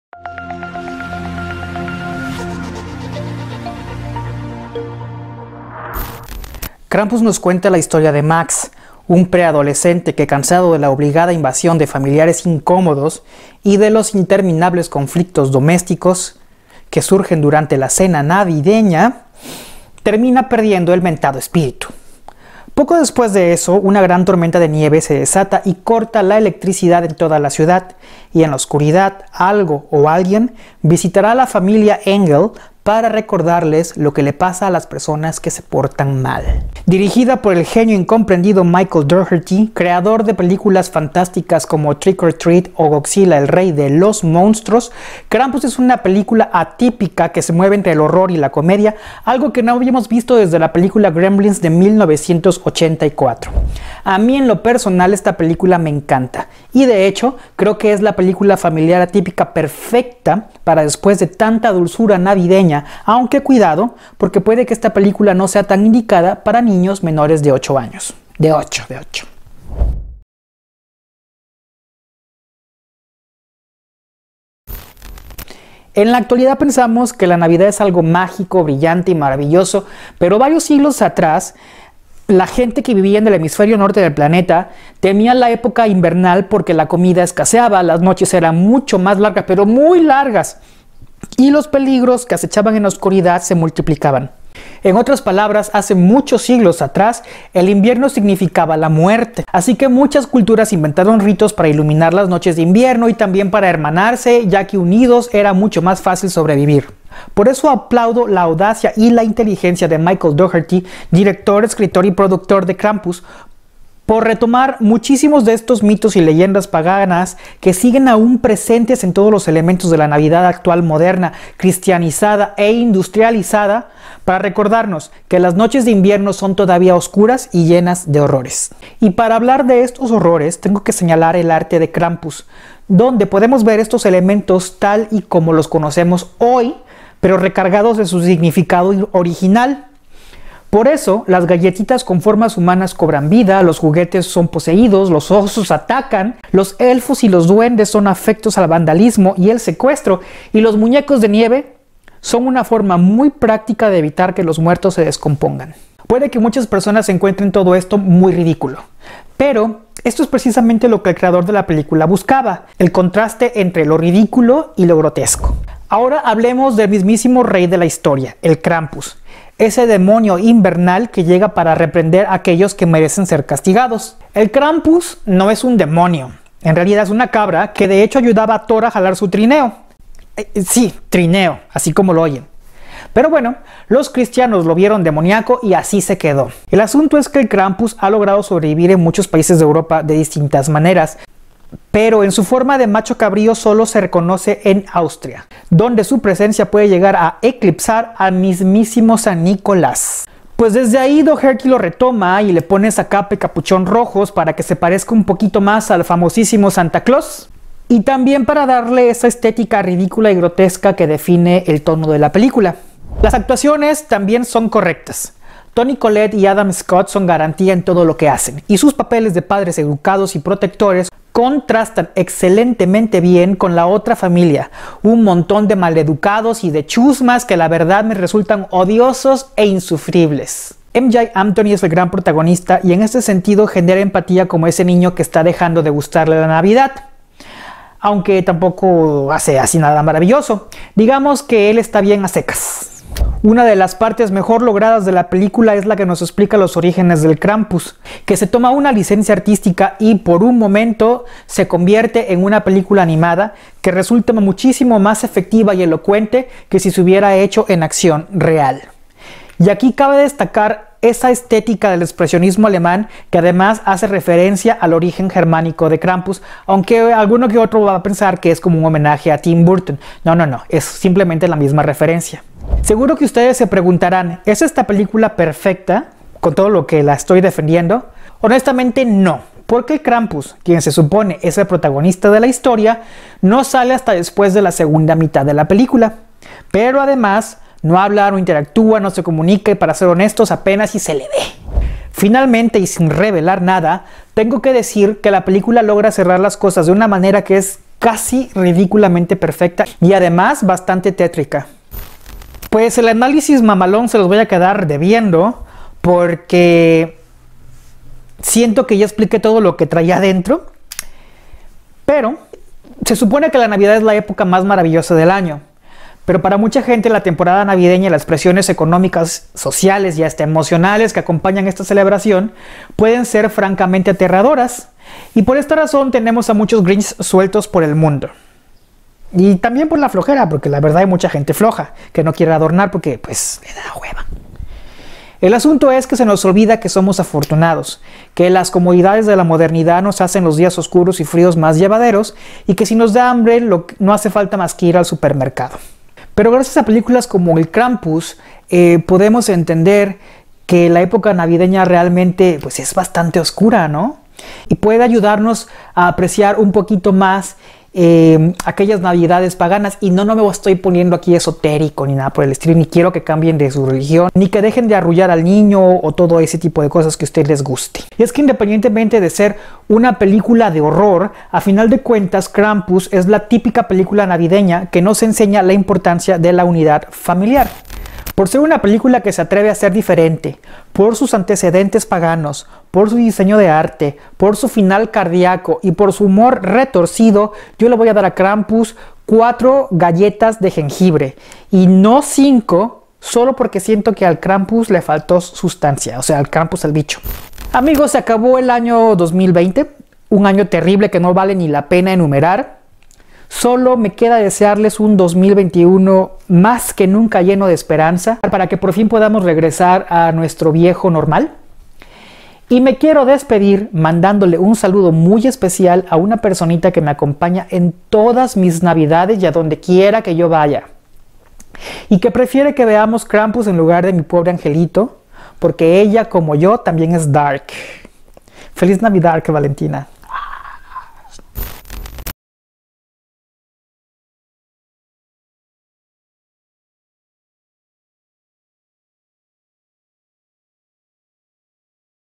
Krampus - Reseña
Una película como pocas: Horror navideño familiar. Algo que no habiamos visto desde Gremlins (1984).  Una obra de culto que estoy seguro que con el tiempo se revindicará en el gusto del público